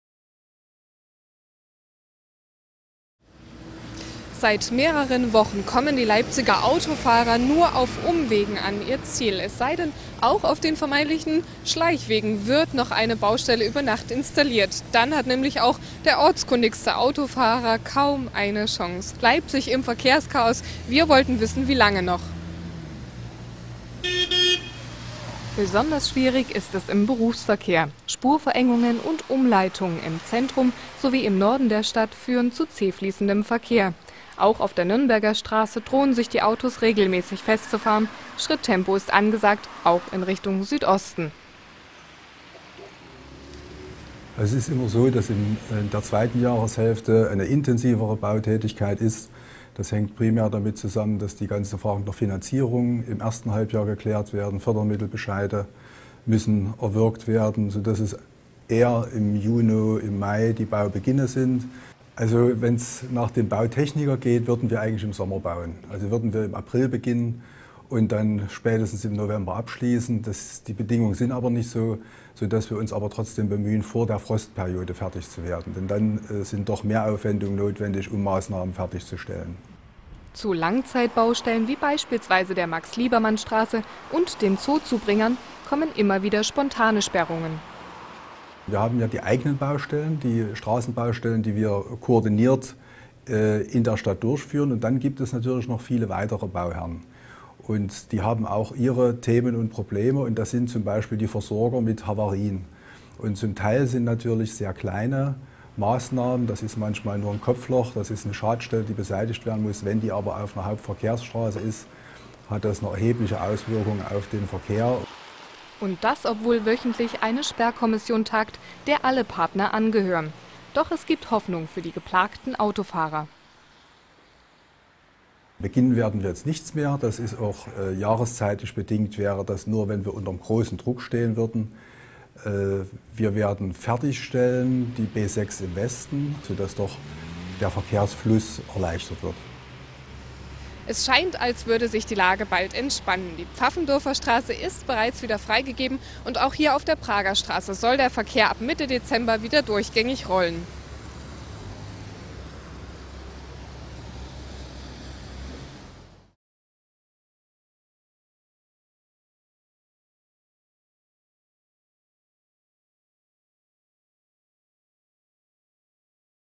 Baustellenchaos in Leipzig bald überstanden
Leipzig, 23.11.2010: Die wochenlangen Sperrungen auf Leipzigs Straßen werden allmählich aufgehoben. Zwar kommt es stellenweise noch immer zu Stau, doch bis zum Beginn der Adventszeit sollen die größten Baustellen verschwunden sein.